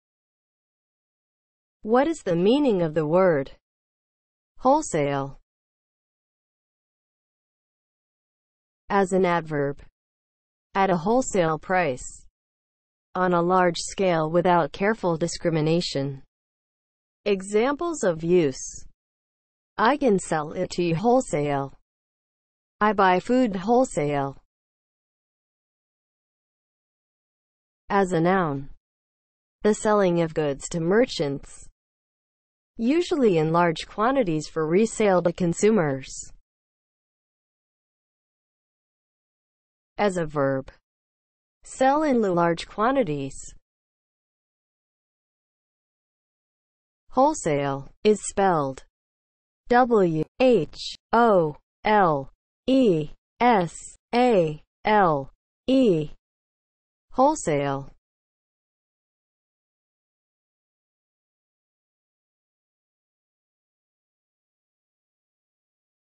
What is the meaning of the word WHOLESALE?
Definition, examples of use and spelling of the word WHOLESALE.

(00:00) Intro
(00:07) As an adverb
(00:26) As a noun
(00:38) As a verb
(00:45) Spelling

As an adverb (00:07):
• at a wholesale price
• on a large scale without careful discrimination

Examples:
• "I can sell it to you wholesale"
• "I buy food wholesale"

As a noun (00:26):
• the selling of goods to merchants; usually in large quantities for resale to consumers

As a verb (00:38):
• sell in large quantities